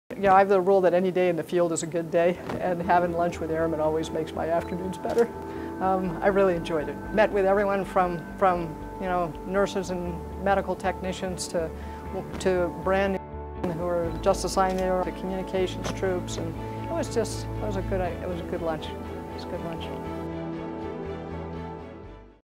Air Force Secretary Heather Wilson visits Holloman
Secretary of the Air Force Heather Wilson visited Holloman this week and enjoyed eating lunch with some of the Airmen.